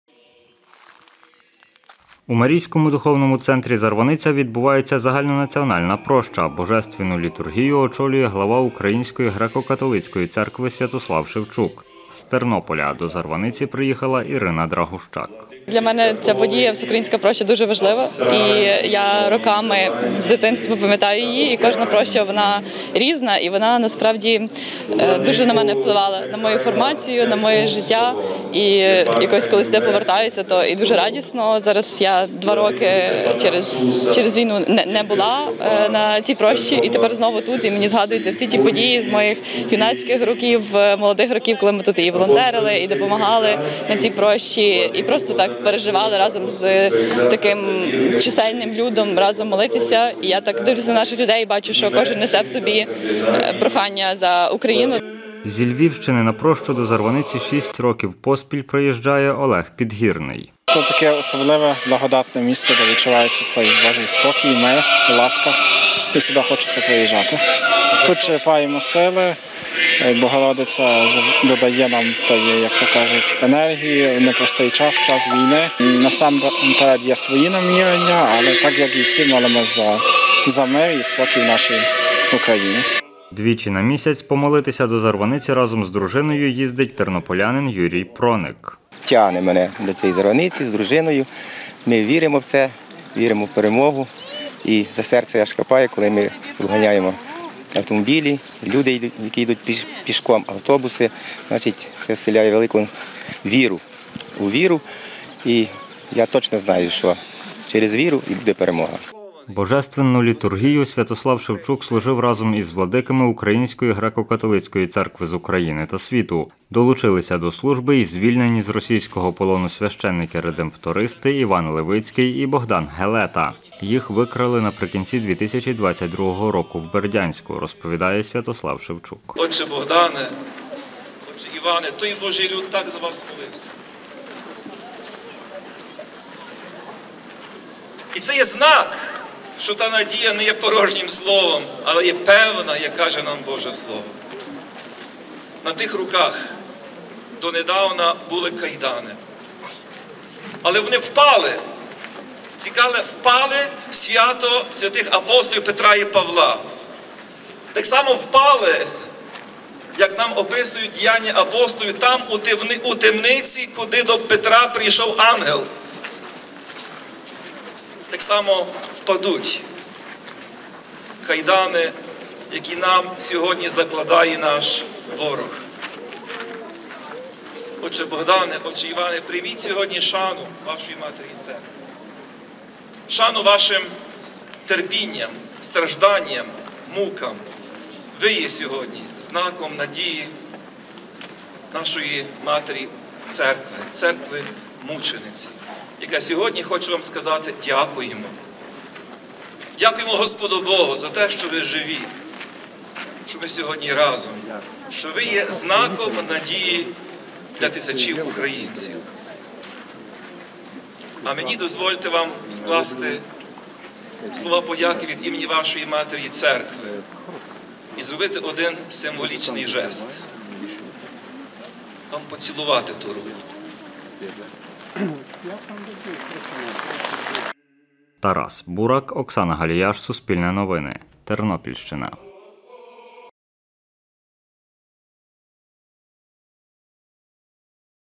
Молилися за перемогу України під час прощі в Зарваниці
Молилися за перемогу України. У Марійському духовному центрі "Зарваниця" відбулася Всеукраїнська проща. Її очолив глава Української греко-католицької церкви Святослав Шевчук. За його словами, на прощу до Зарваниці прибули майже 30 тисяч людей зі всієї України. Більше – в сюжеті.